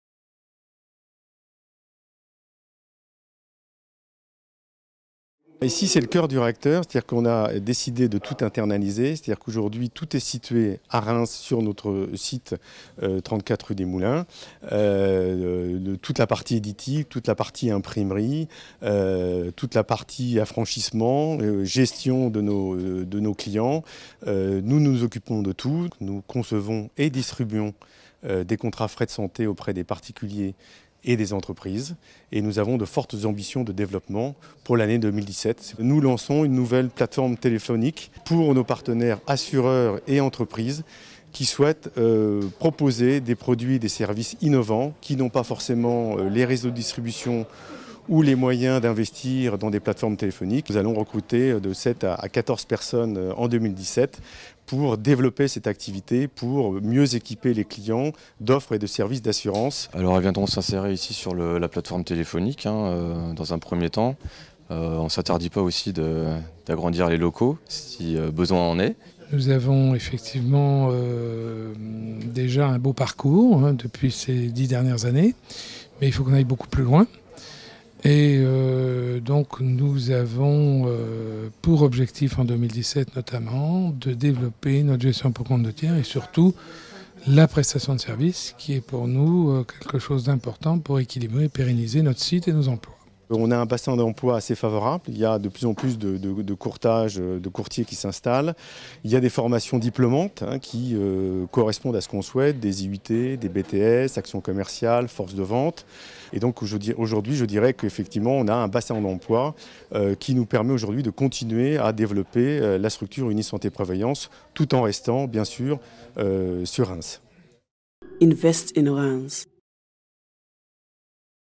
Unisanté Prévoyance confirme l'attractivité de Reims dans l'assurance
Crédit : KILKOA pour Invest in Reims